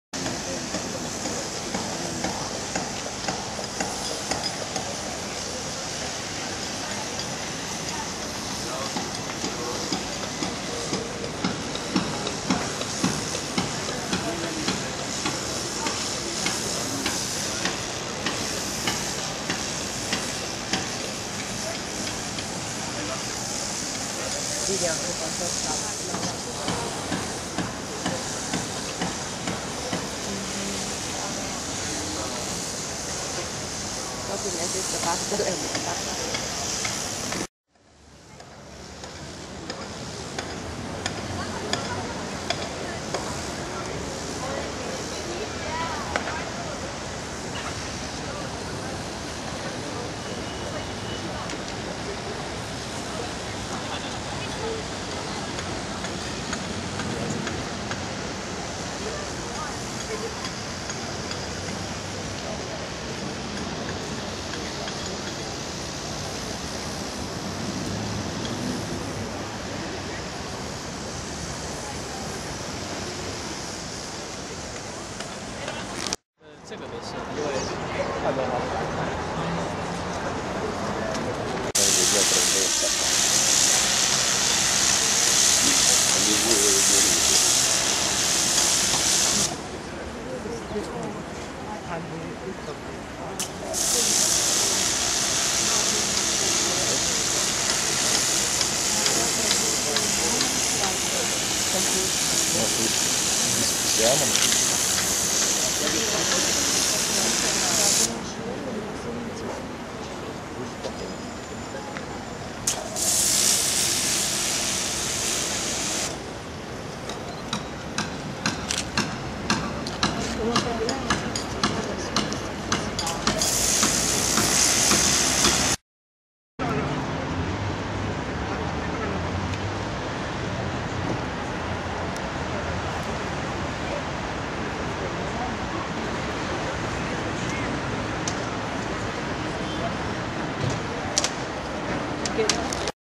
Roman Colosseum April 2011
A short trip to the Roman Colosseum in April 2011